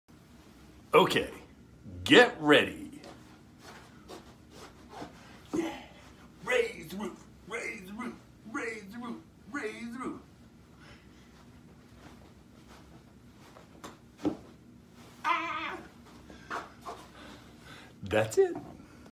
Matthew Frey of US Event Photos Raises the Roof
Matthew Frey, photographer with US Event Photos dances in his hotel room.